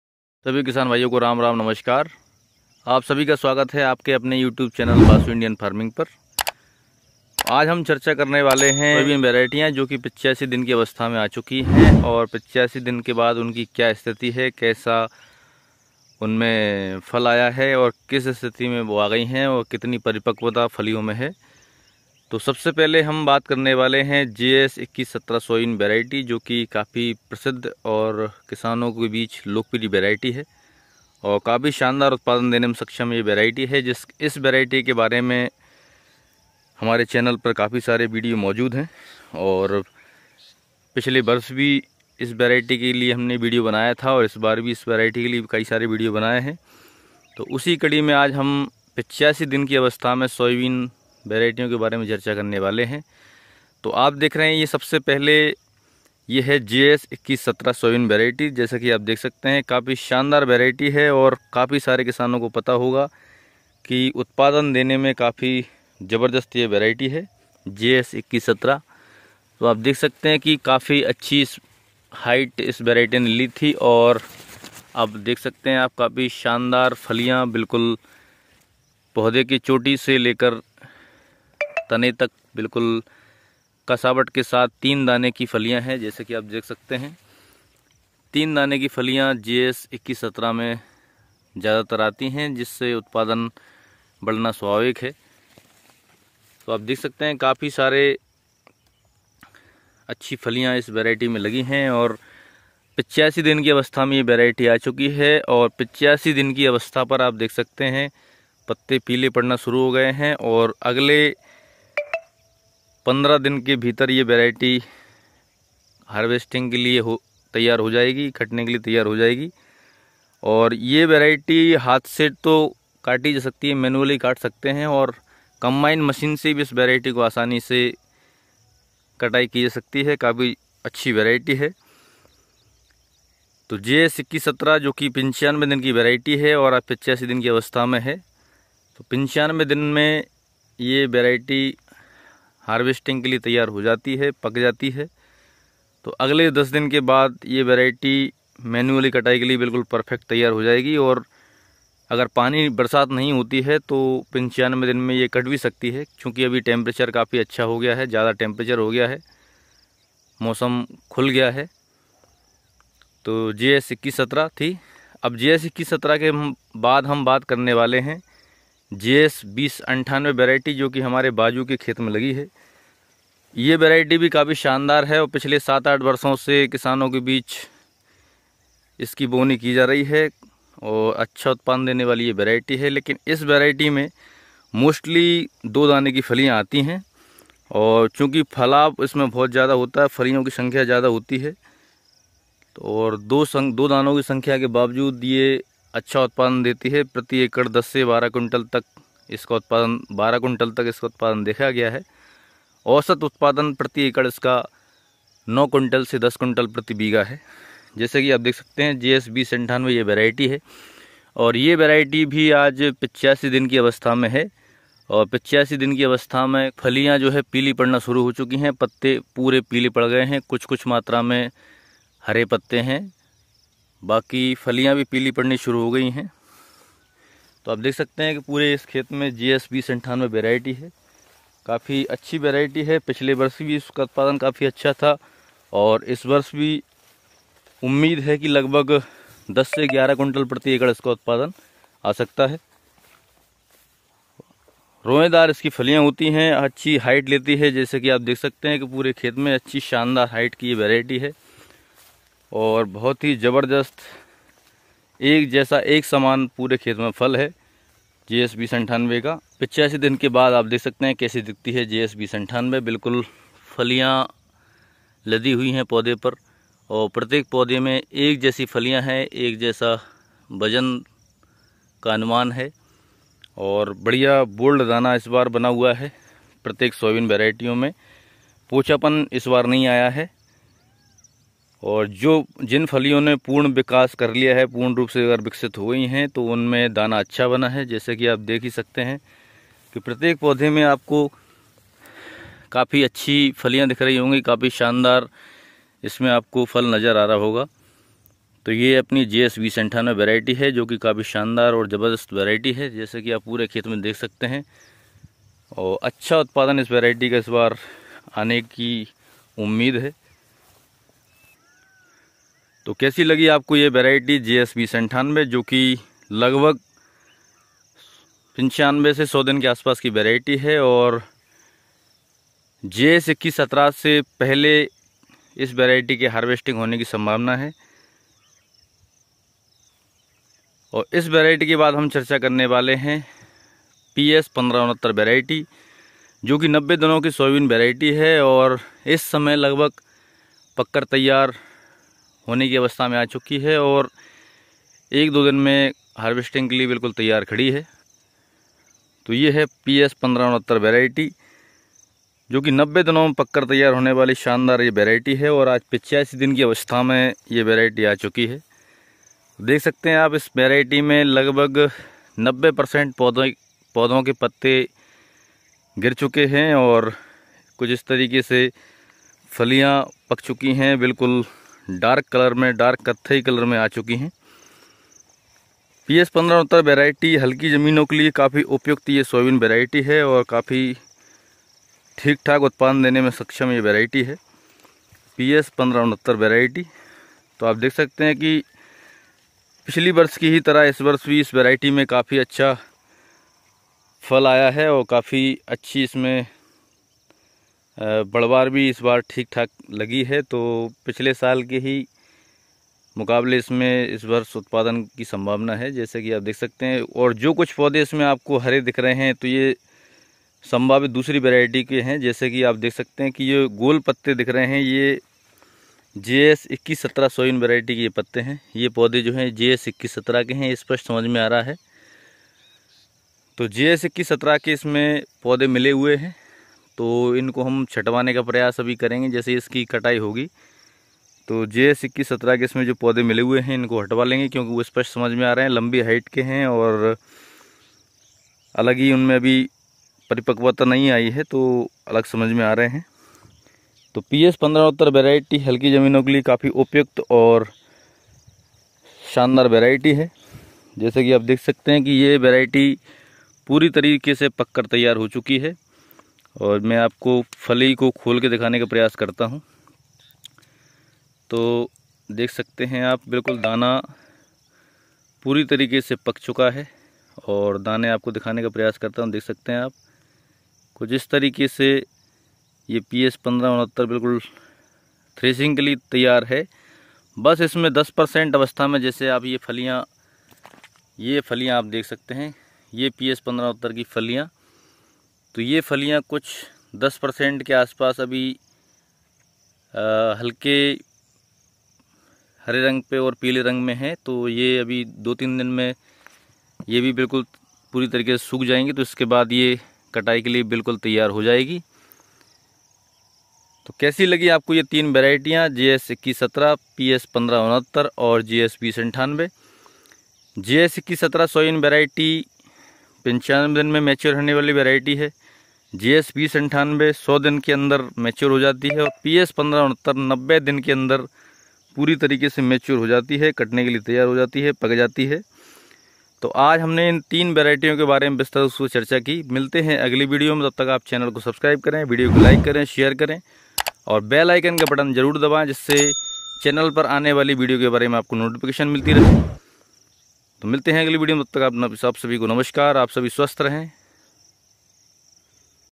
85 दिनों की अवस्था मै तीन सोयाबीन किस्में, क्या ये किस्में किसानों को मालामाल कर पाएंगी
नमस्कार किसान भाइयों 85 दिन की अवस्था मै सोयाबीन किस्मों का वीडियो आपके लिए अपने चैनल पर आ गया है